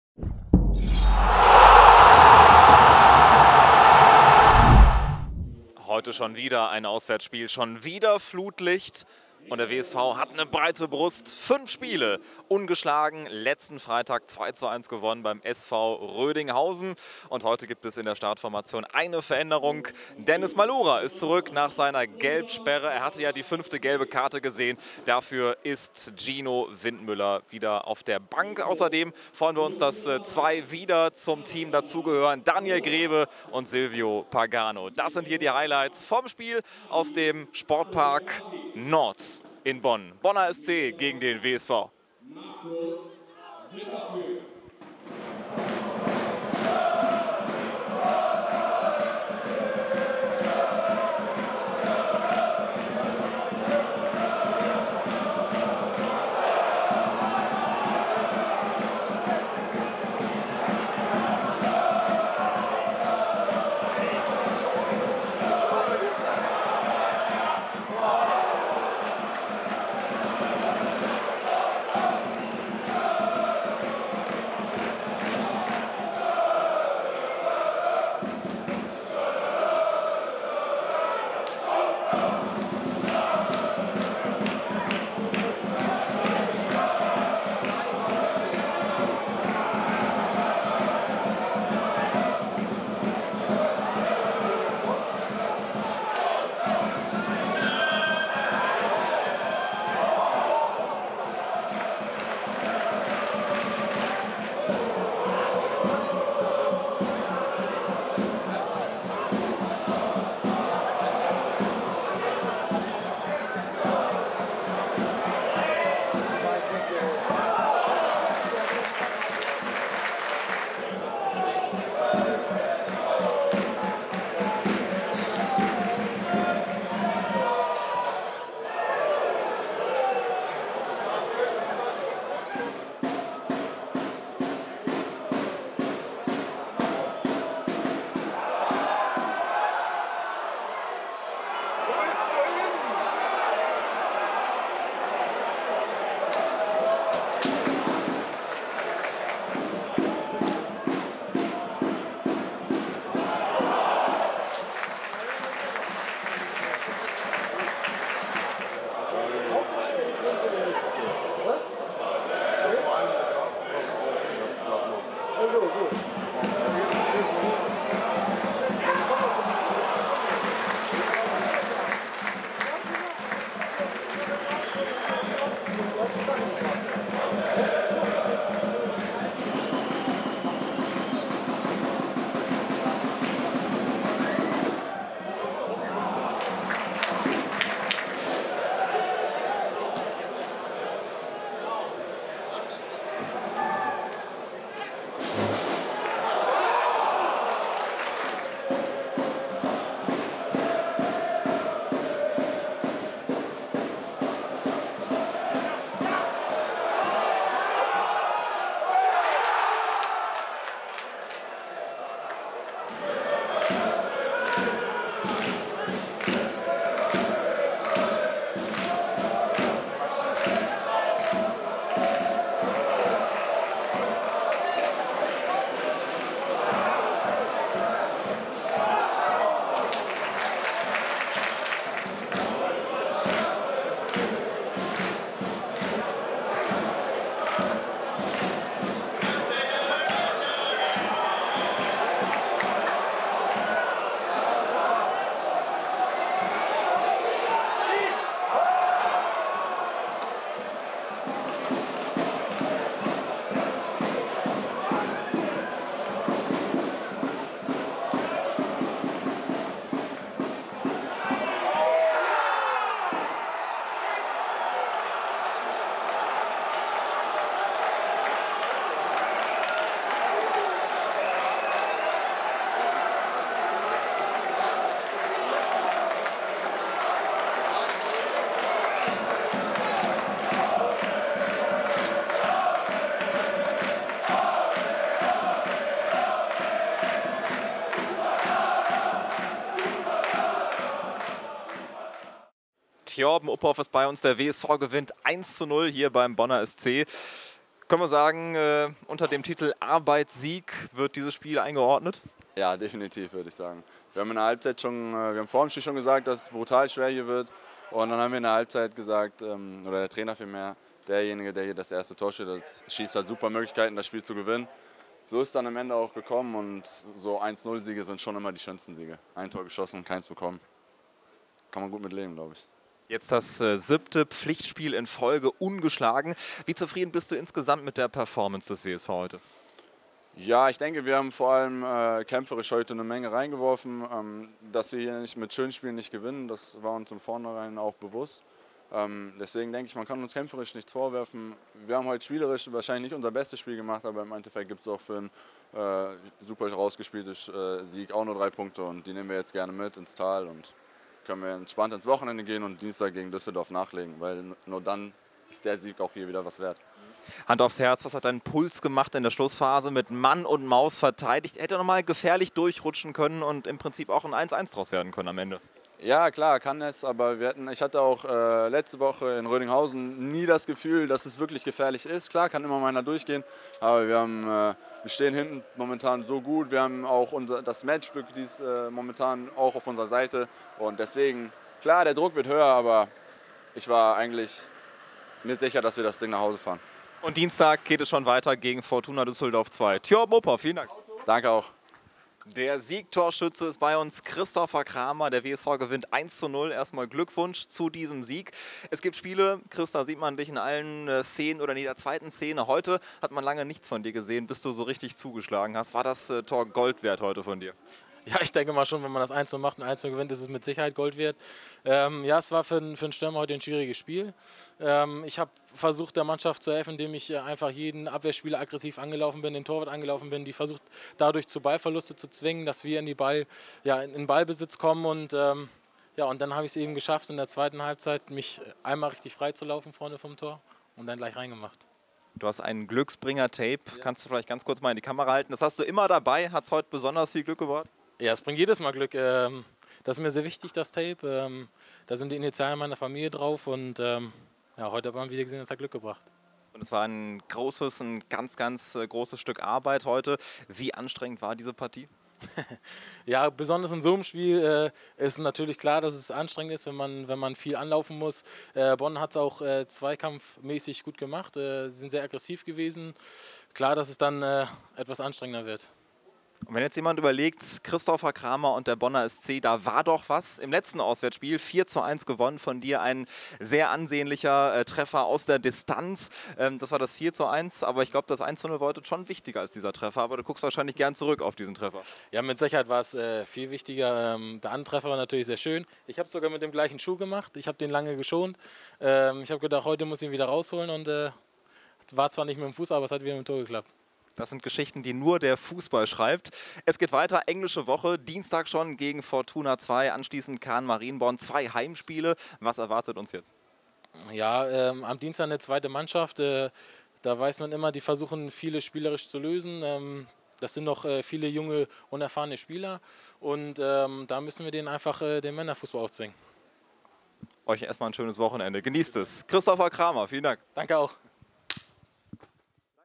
WSV-TV: Bonner SC - Wuppertaler SV 18/19
WSV seit sieben Pflichtspielen ungeschlagen
Das nächste Auswärtsspiel, schon wieder bei Flutlicht. Nach dem 2:1 beim SV Rödinghausen traten die Rot-Blauen im Sportpark Nord beim Bonner SC an. Gegen den Club aus der früheren Bundeshauptstadt änderte Chefcoach Adrian Alipour das Team auf einer Position. Für Gino Windmüller kehrte der gelbgesperrte Dennis Malura zurück in die Startelf.

Die erste Großchance für den WSV in der Anfangsviertelstunde: Nach Vorarbeit von Alabas verpasst Erwig-Drüppel das BSC-Gehäuse nur knapp – drüber (13.)! Nach einigen Kontergelegenheiten für die Gastgeber kombinierten sich Manno und Kramer gefährlich vors Tor (33.). Eine abwechslungsreiche erste Hälfte endete dennoch torlos.

Mit dem Seitenwechsel änderte sich an diesem Schlagabtausch nichts. Nach einer Stunde Jubel im Heimblock: Doch Bors traf nach Zuspiel von Somuah nur das Außennetz (60.). Nur wenige Zeigerumdrehungen später der Lucky Punch für den WSV. Erwig-Drüppels Flanke bereitete mustergültig Kramers 11. Saisontor vor (65.). Kurios: Er traf mit demselben Schuh wie schon beim letzten Auswärtsspiel in Bonn, als er in der 89. per Distanzschuss das 4:1 erzielte. Auch wenn die Bonner in der Schlussphase drückten und die Defensive der Rot-Blauen permanten unter Druck setzen, konnte der WSV das 1:0 verteidigen.

Fazit: Der Wuppertaler SV ist nun bereits seit sieben Pflichtspielen ungeschlagen. Das 1:0 in Bonn kann in der Kategorie „Arbeitssieg“ eingeordnet werden – allerdings auch einer, den das Team durch Einsatz und Willen verdient hat. Weiter geht es schon am kommenden Dienstag mit dem Heimspiel gegen die U23 von Fortuna Düsseldorf (Stadion am Zoo, Anstoß 19:30 Uhr).